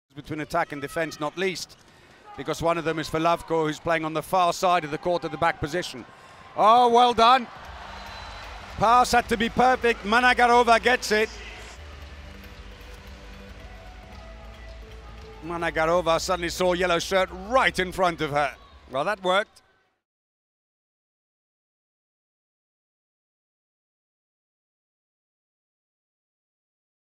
Amazing catch and goal by Managarova | Women's EHF EURO 2018
Iuliia Managarova successfully receives an accurate pass and wastes no time to beat Katarina Tomasevic.